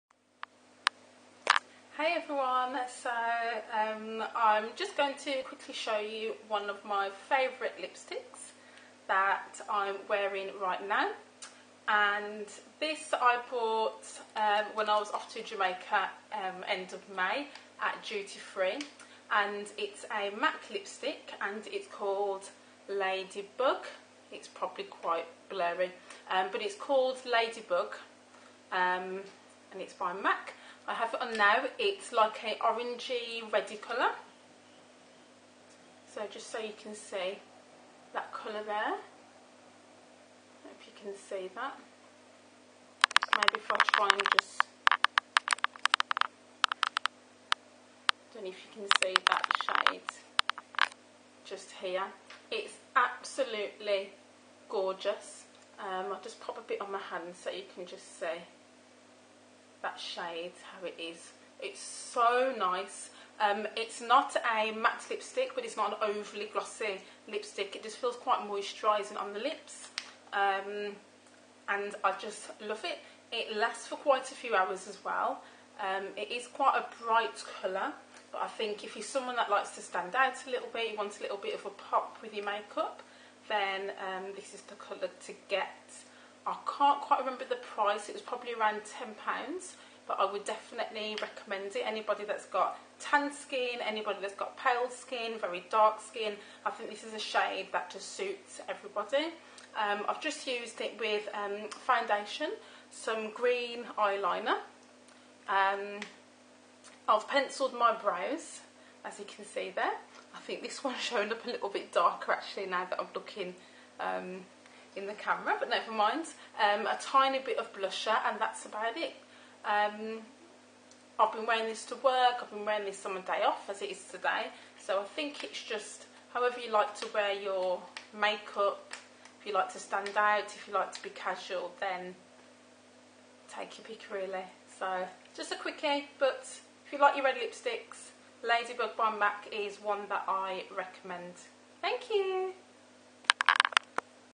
Mac lipstick - Lady Bug
I show you my favourite Mac lipstick atm, and also you can see pictures on my blog and more info. I love this lipstick colour as its like a orangy, red colour and really moisturising.